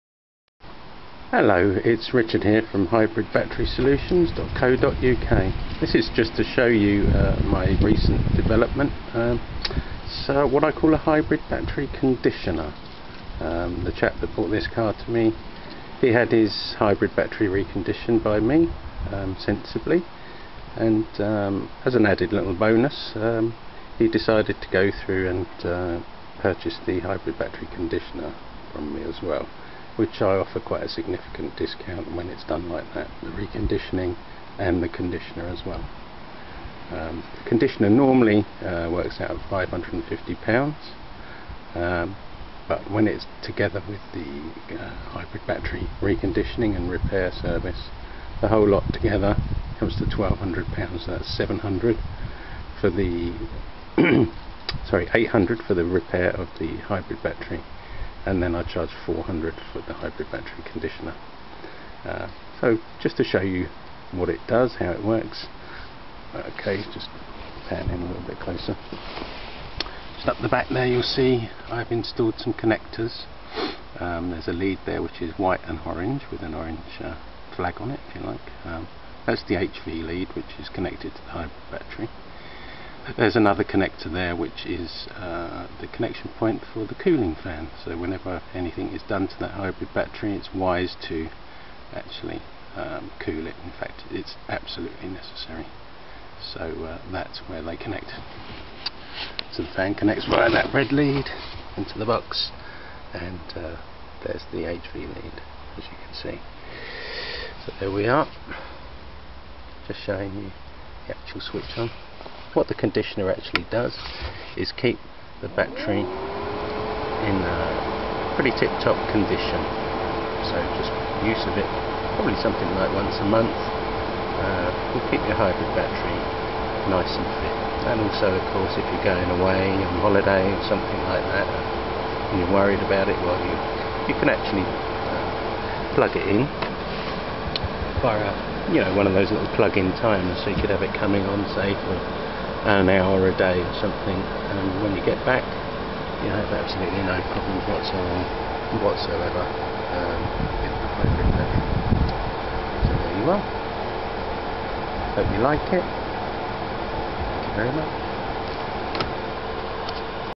Lexus Hybrid Battery conditioner
A brief demonstration of my recent development - Recommended for all those who wish to give their GS450h the best chance of avoiding Hybrid Battery Failure - The prices on this video are outdated, the new total price for repair and reconditioning is £745 and I offer a discounted price of £500 for a conditioner (normal price £550), bringing the total to £1245.